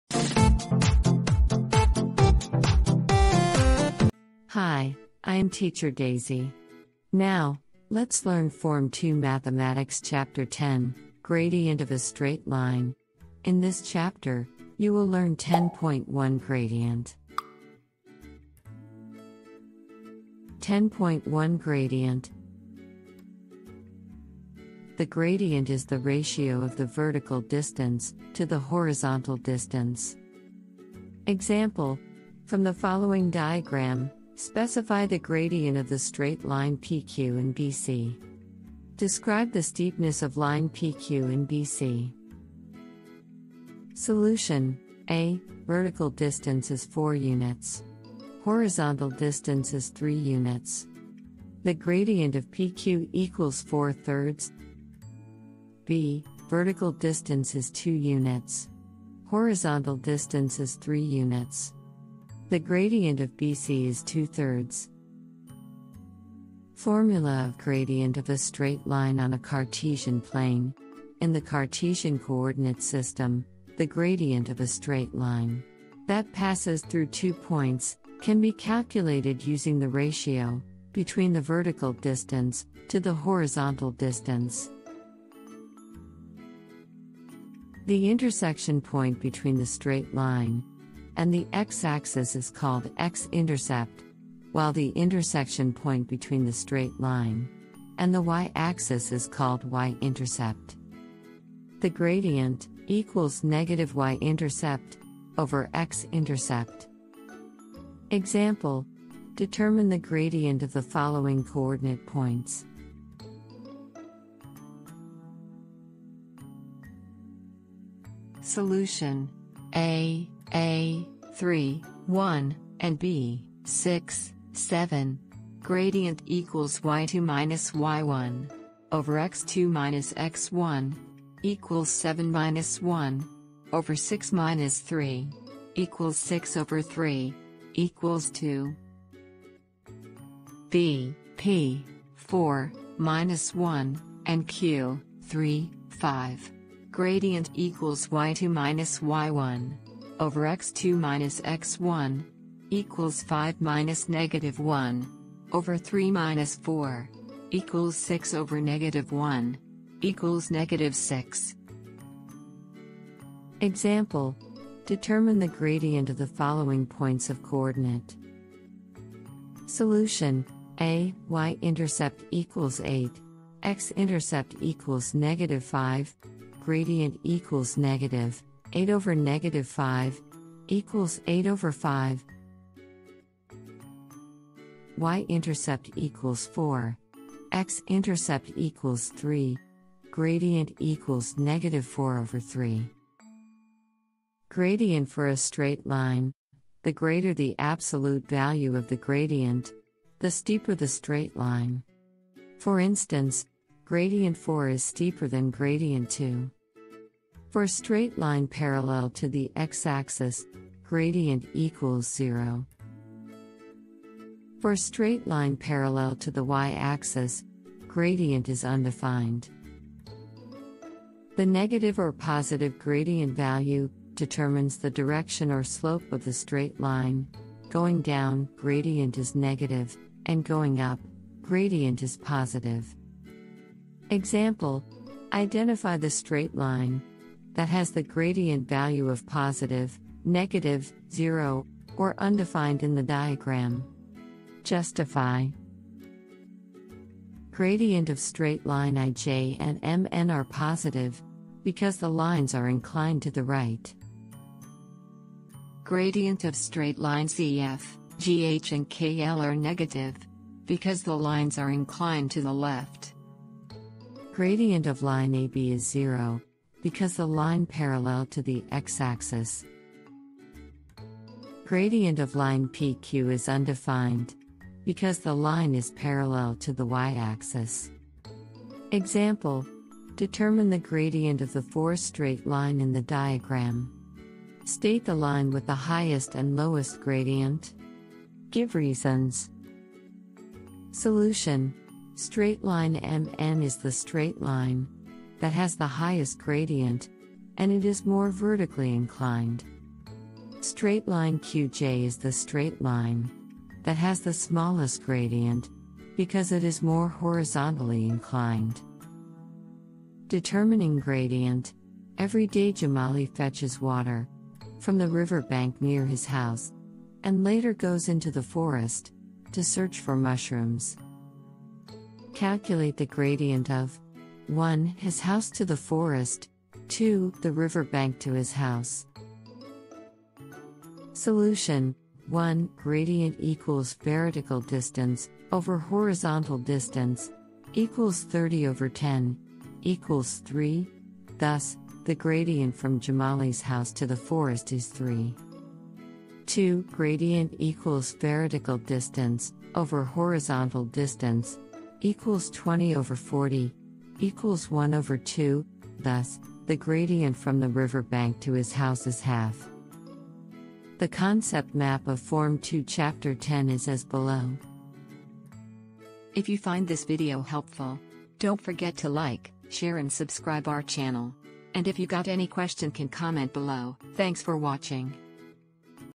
PT3 KSSM Mathematics Form 2 (Gradient of a Straight Line) Chapter 10 Complete Revision
0:00 intro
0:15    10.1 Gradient